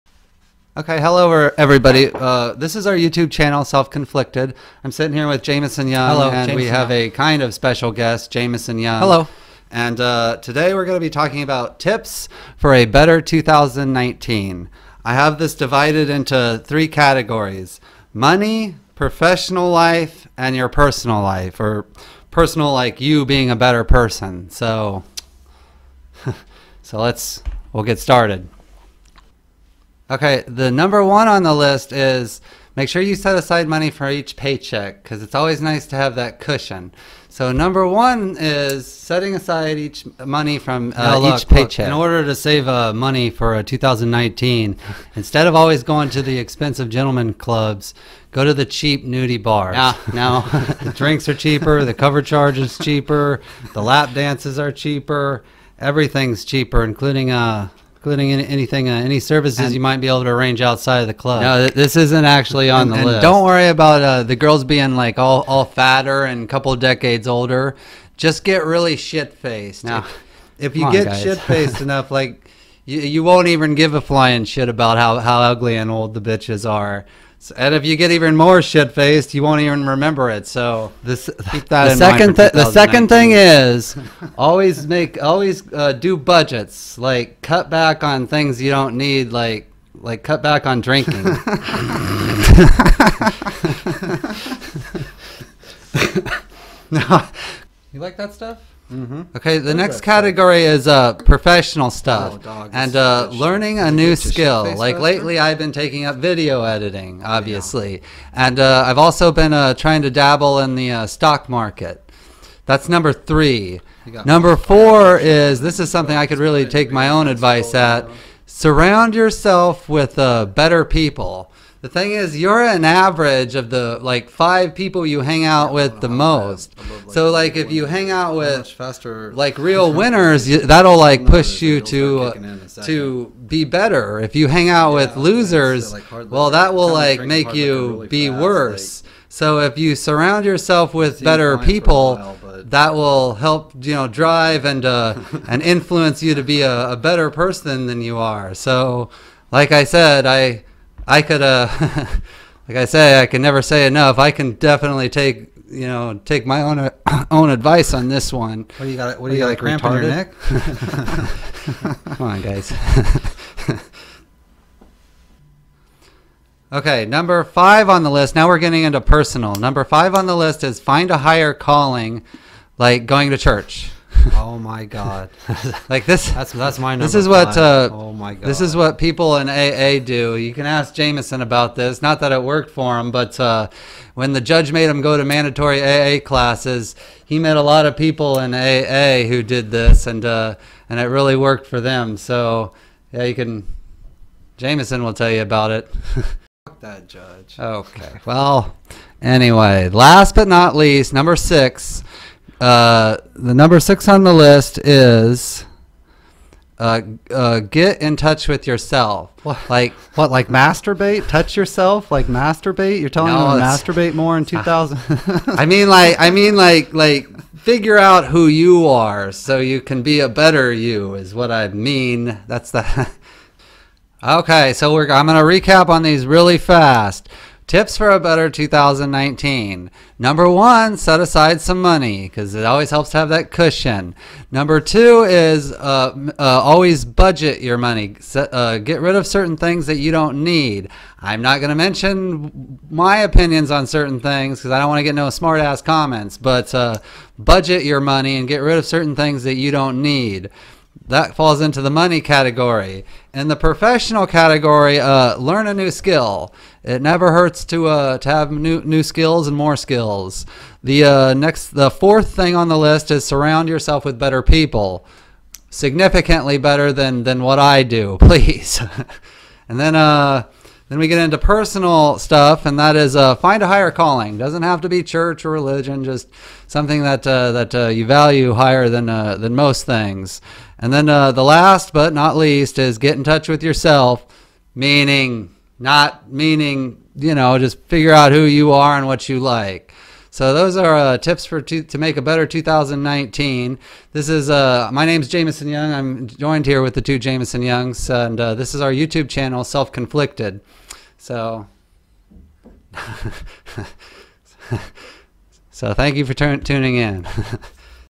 Tips for a Better 2019
Jameson, Jameson, and Jameson Talk about how to have a better 2019.